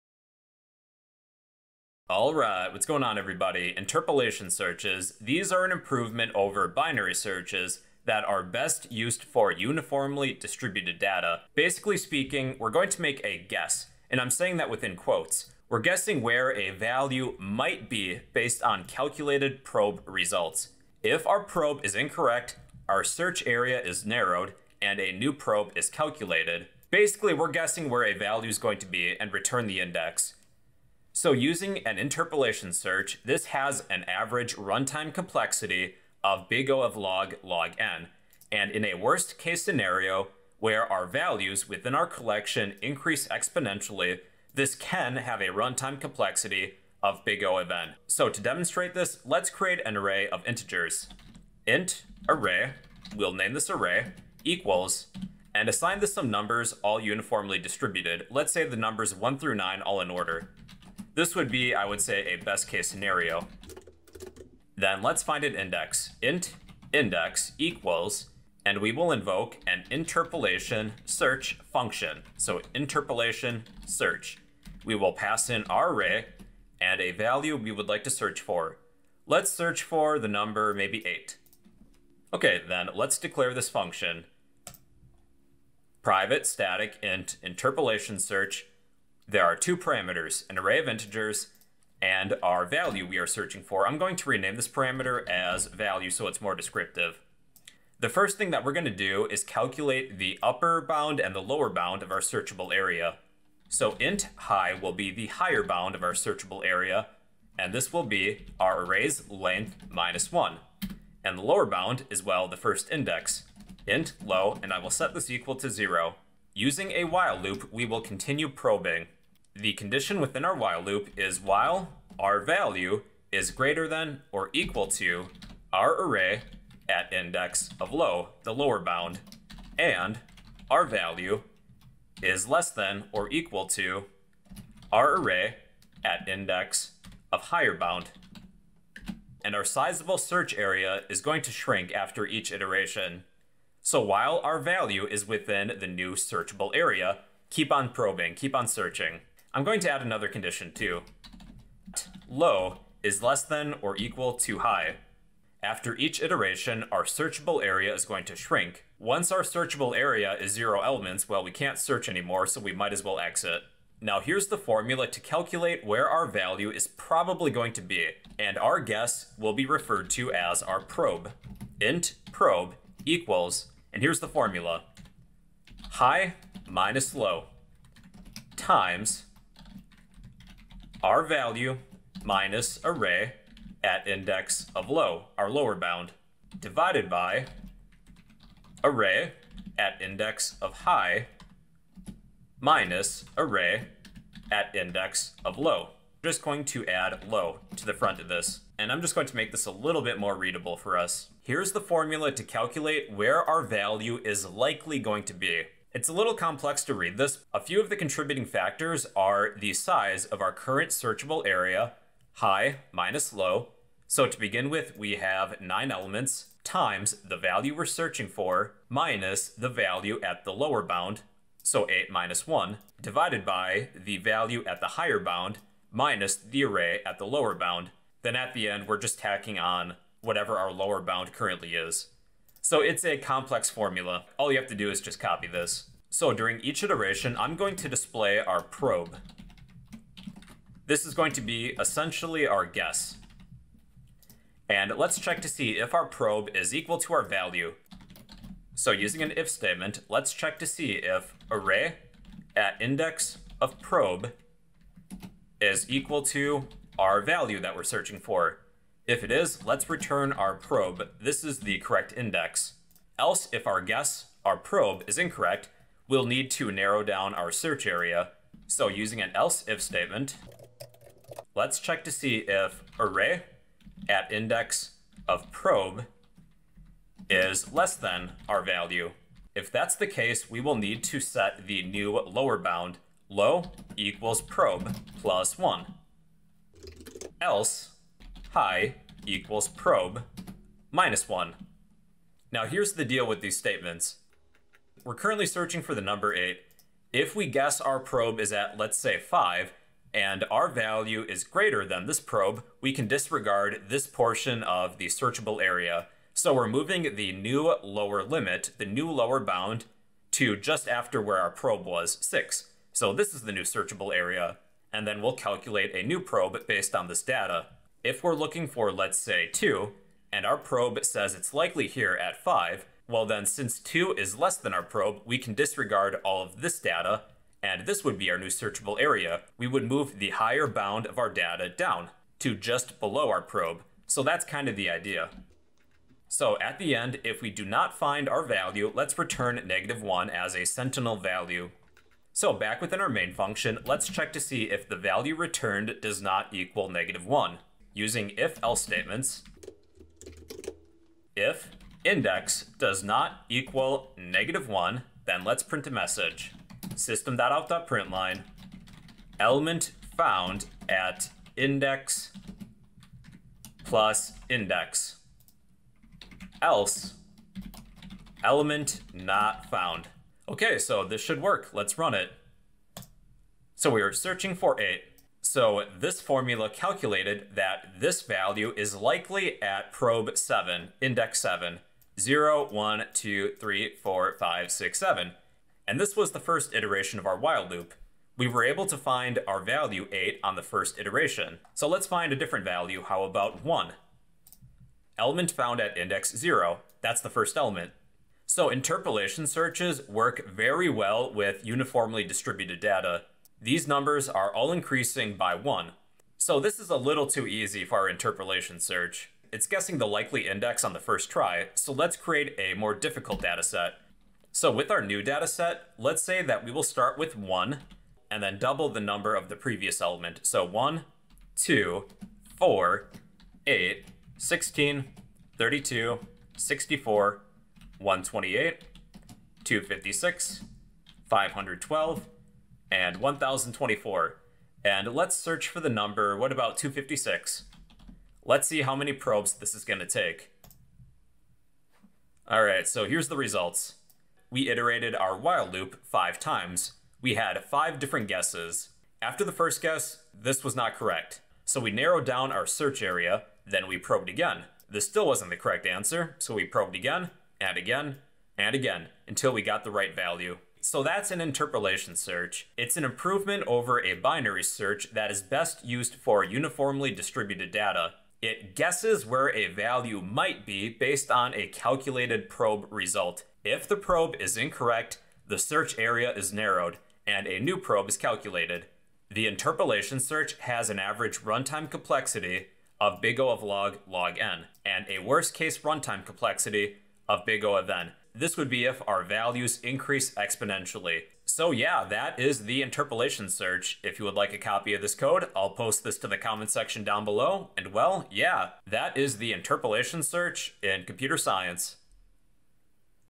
Learn Interpolation search in 8 minutes ❓
Interpolation search data structures and algorithms tutorial example explained